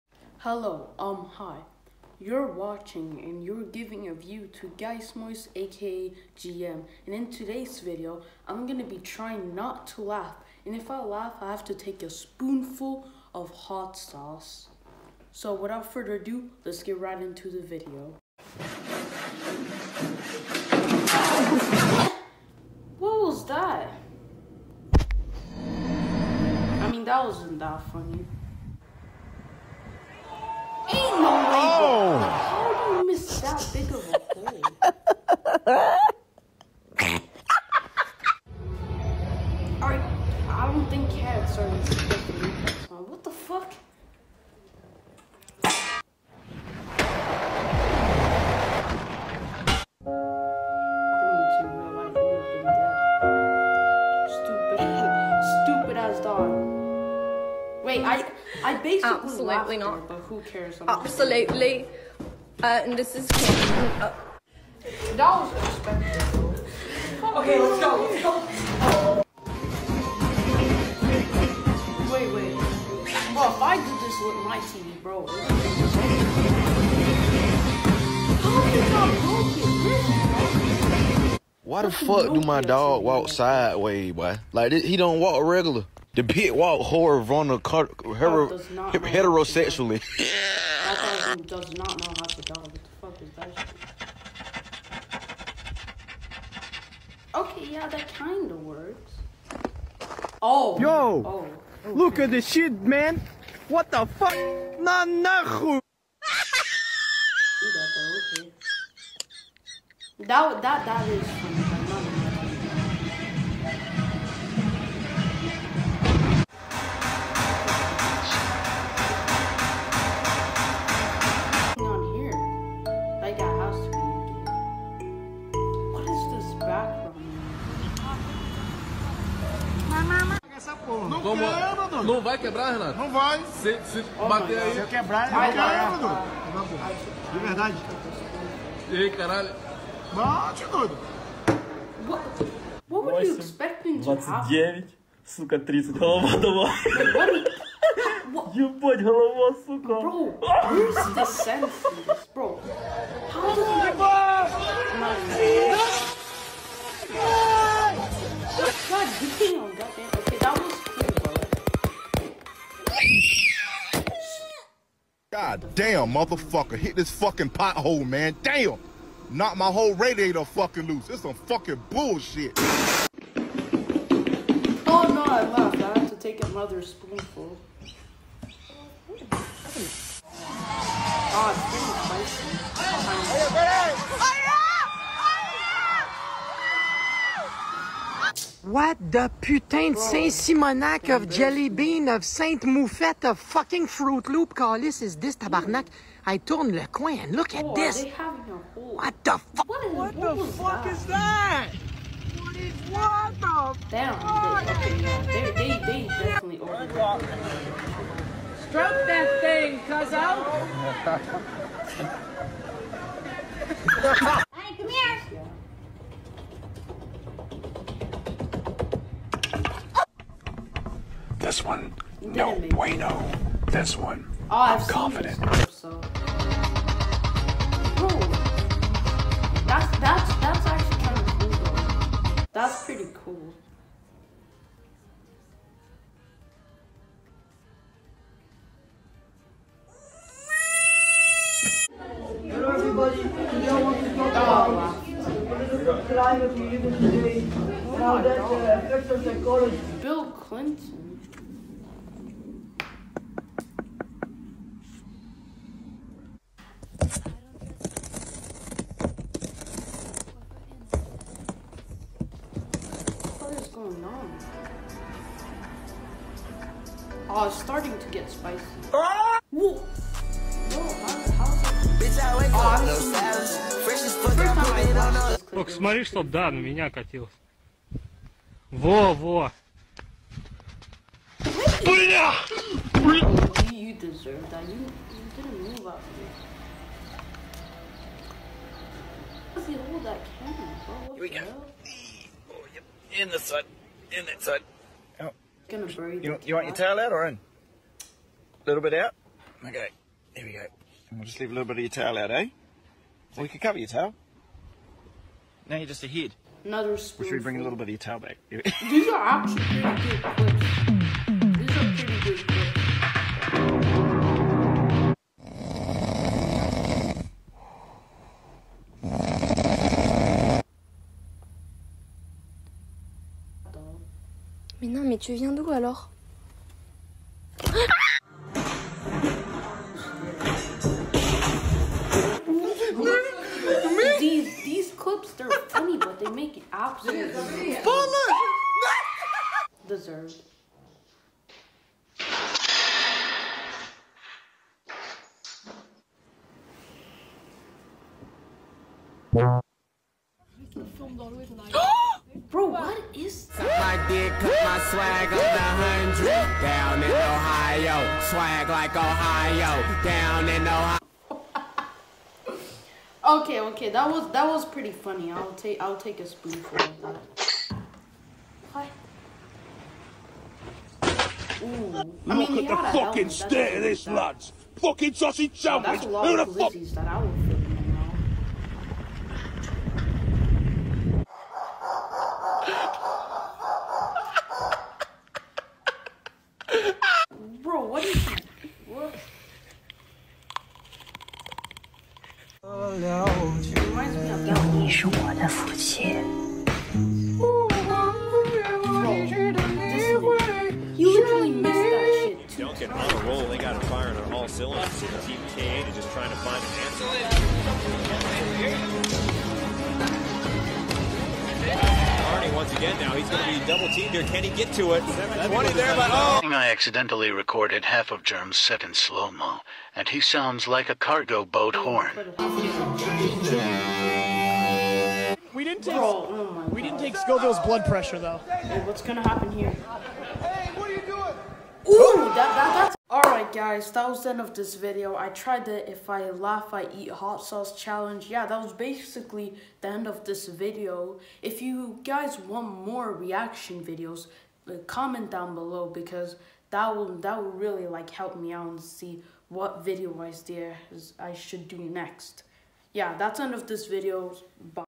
If I laugh I eat hot sauce
Ay this is my first video that isnt a YouTube Short so please like-
I know that you couldnt hear me so clear so next time I Do video like this imma talk louder